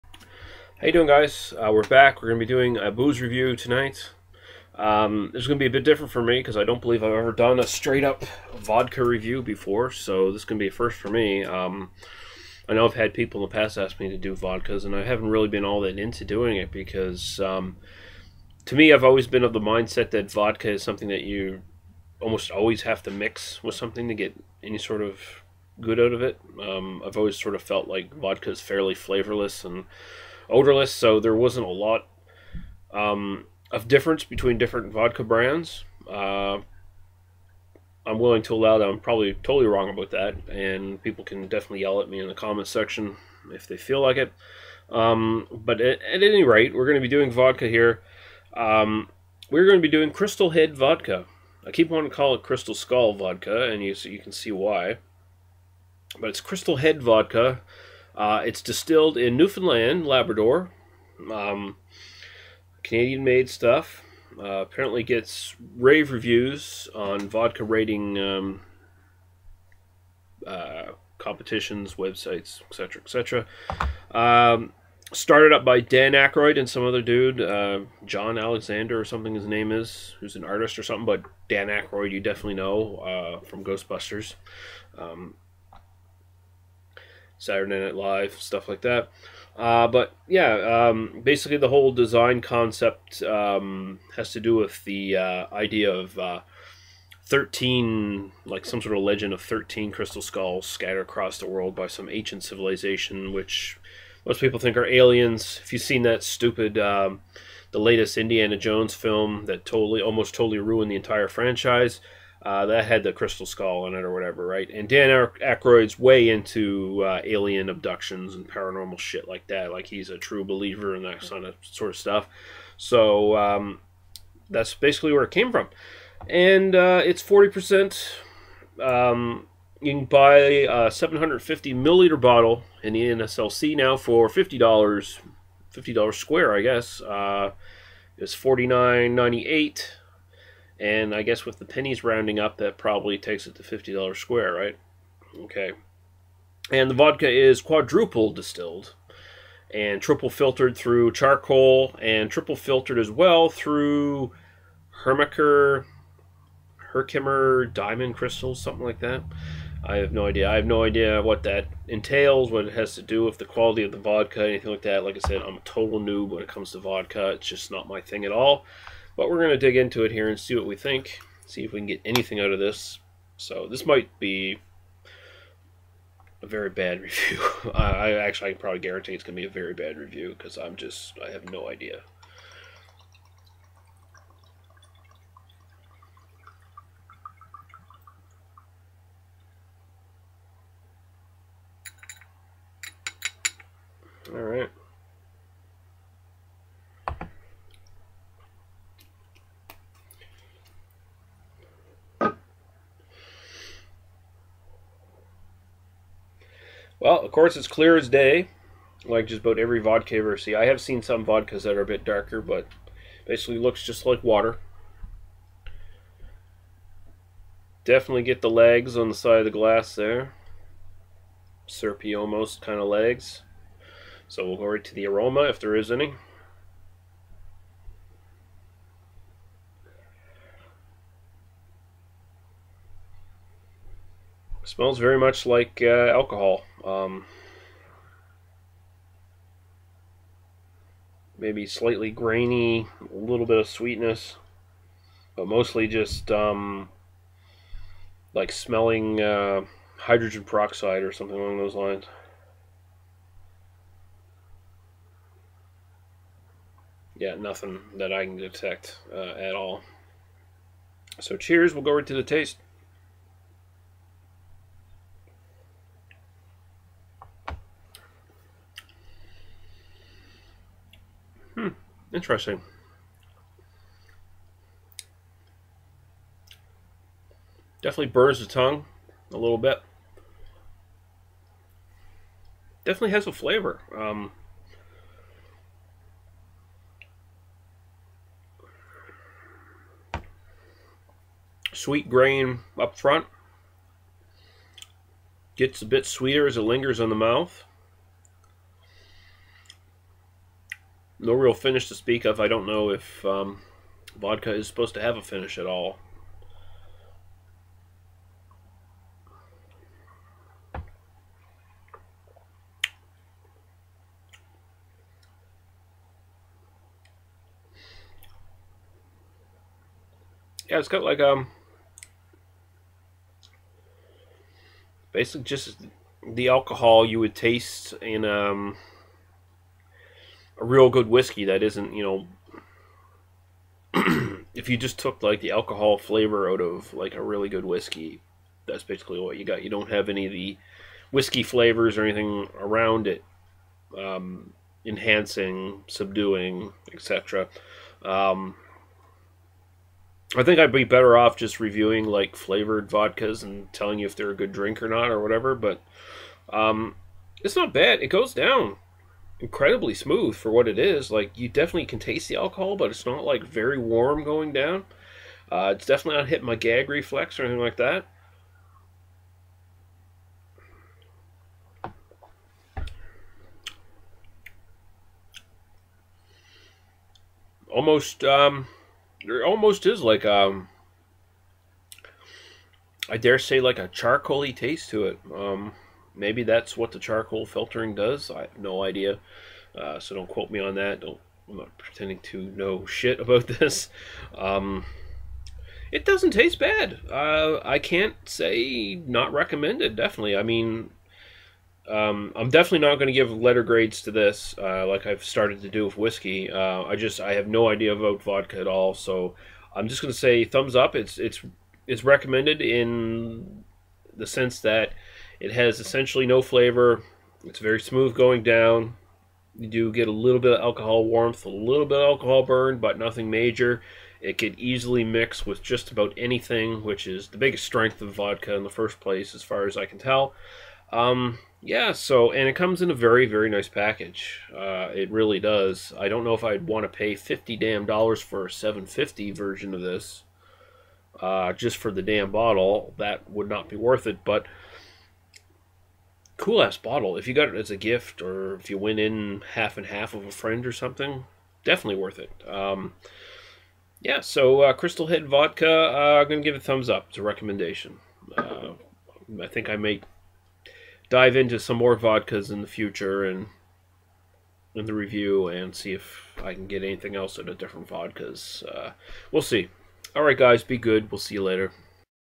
Hougly Booze Review: Crystal Head Vodka.
I take a shot at reviewing straight vodka. I have the colour filter on there to hide the grainy shit look of the video a bit, as I'm still having webcam issues.